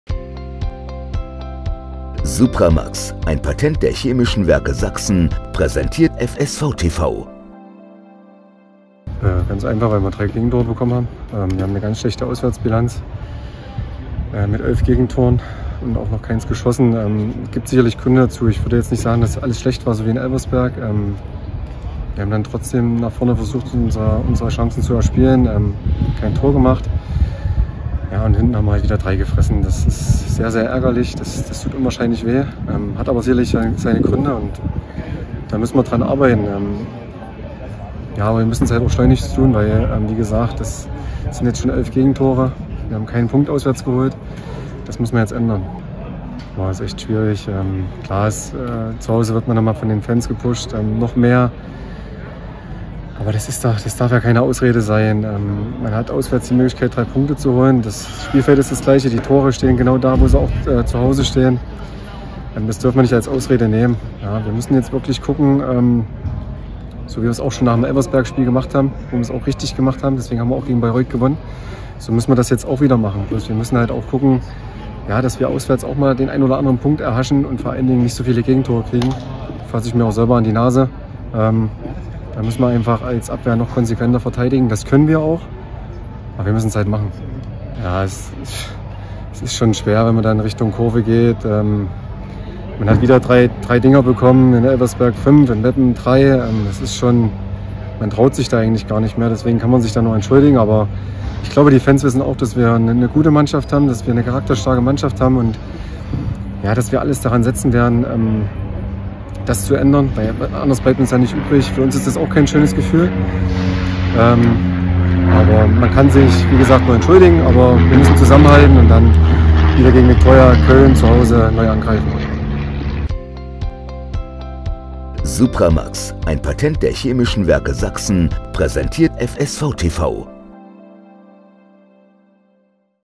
FSV TV | NACHSPIELZEIT Davy Frick nach der Auswärtsniederlage beim SC Verl
Der FSV bleibt ein gern gesehener Gast in den Stadien der 3. Liga. Auch beim bisherigen Tabellenletzten SCVerl gab es nichts zu holen. Die Partie in der Paderborner Home Deluxe Arena ging klar mit 0:3 (0:2) verloren.

Tore:
1:0 Grodowski (12.), 2:0 Akono (27.), 3:0 Stellwagen (90.+4)

SC Verl:
Thiese – Ochojski, Mikic, Stöcker/gelb, Stellwagen – Sapina – Baack, Corboz – Grodowski (71. Biondic), Wolfram (76. Probst), Akono (90.+1 Dag)

FSV Zwickau:
Brinkies – Butzen, Kusić, Frick, Herrmann – Göbel (78. von Schroetter), Jansen/gelb, Könnecke (54. Gómez), Coşkun (54. Agbaje) – König (70. Krüger), Baumann (70. Löhmannsröben)

Zuschauer: 722 (160 Gäste)

Schiedsrichter: Patrick Alt (Illingen/Saarland)